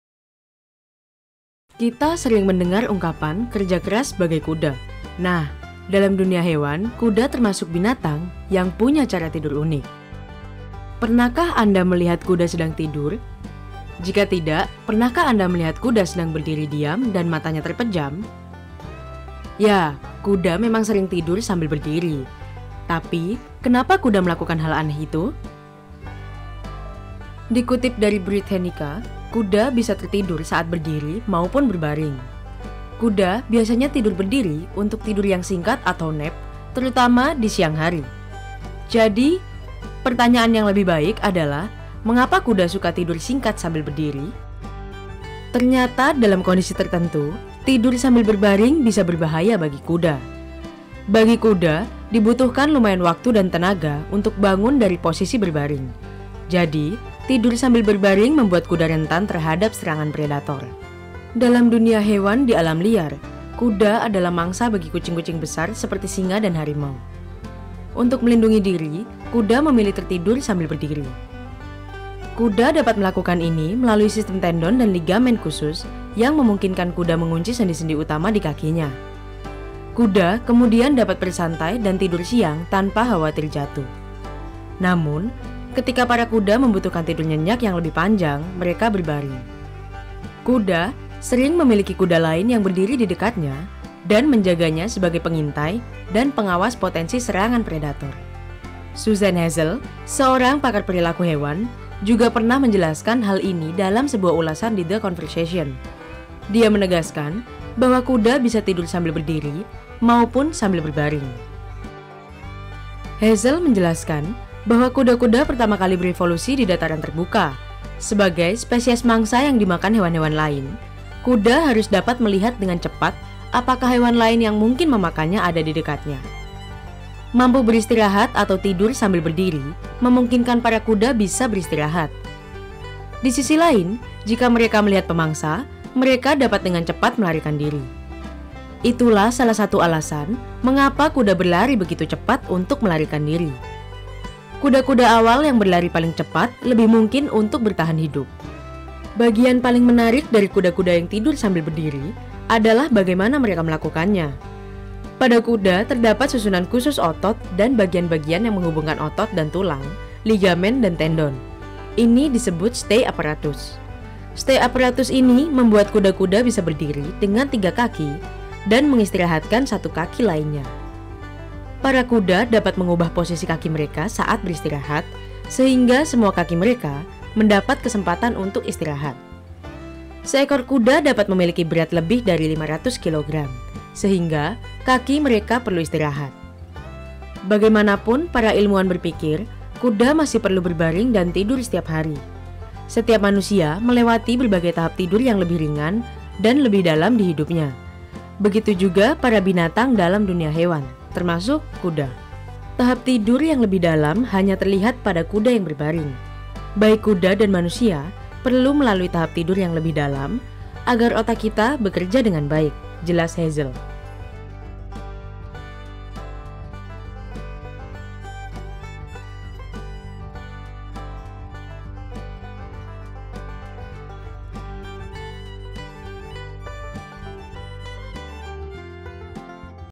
Mengapa Kuda si Pekerja Keras Suka Tidur sambil Berdiri ?
Dalam dunia hewan, kuda termasuk binatang yang punya cara tidur unik. Pernahkah Anda melihat kuda sedang tidur? Jika tidak, pernahkah Anda melihat kuda sedang diam dan matanya terpejam? Ya, kuda memang sering tidur sambil berdiri. Tapi kenapa kuda melakukan hal aneh itu?

Ternyata, dalam kondisi tertentu, tidur sambil berbaring bisa berbahaya bagi kuda. Bagi kuda, dibutuhkan lumayan waktu dan tenaga untuk bangun dari posisi berbaring. Jadi, tidur sambil berbaring membuat kuda rentan terhadap serangan predator.

Riset dan video oleh Utomo Priyambodo
Narator oleh Silvia Triyanti Luis
Editor oleh Aga Akbel Pratama

Redaksi:
Kelurahan Gelora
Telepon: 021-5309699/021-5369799